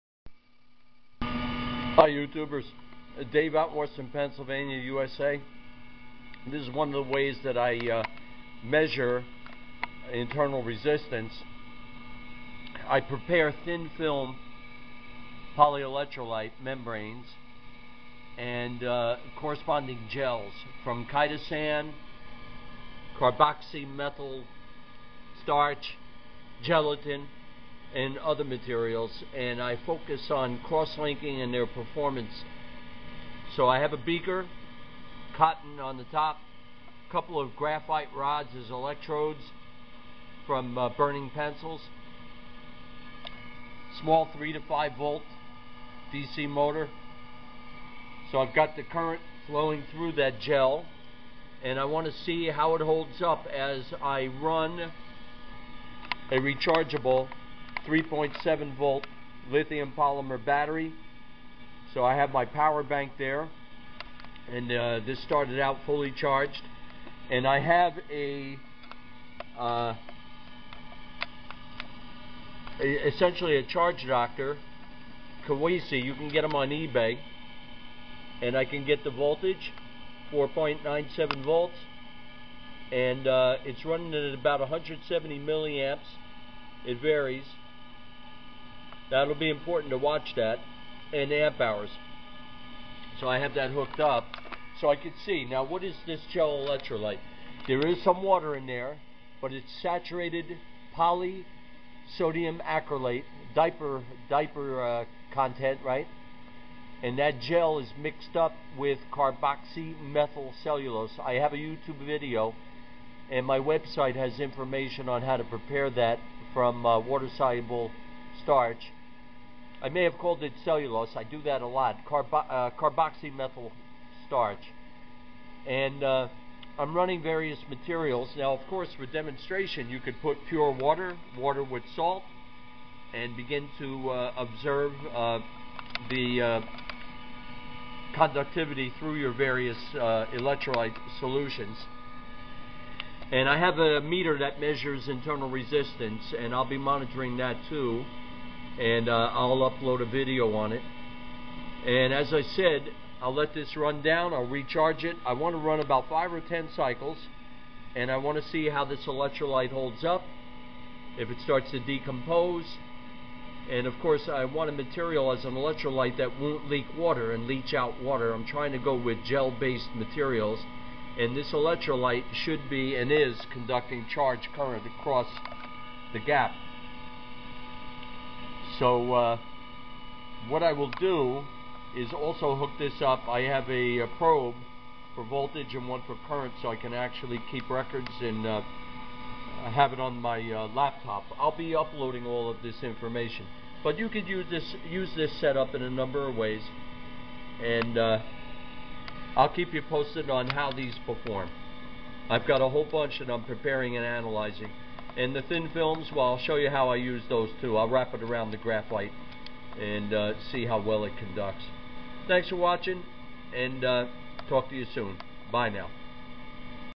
Batteries/ Gel Electrolyte/ Bench-Scale Testing
I conduct research on the preparation, characterization and performance of thin film/ gel polyelectrolytes from renewable sources such as chitosan, carboxymethyl starch, gelatin, etc. The degree of chemical /physical cross-linking (mechanical stabilization) is an important criterion. I show in this video the use of a fully0charged 3.7V lithium polymer battery providing current flowing through a beaker with two graphite electrodes ( from pencils) to run a 3-5V DC motor. I have a Keweisi (eBay) meter interfaced providing voltage, current , time of a given run and mAh. I will run a number of cycles in trying to estimate how the gel electrolyte performs. In this run, the gel has a minimal amount of water present. A cotton plug retains moisture and also holds the graphite electrodes. Fu videos will show Vernier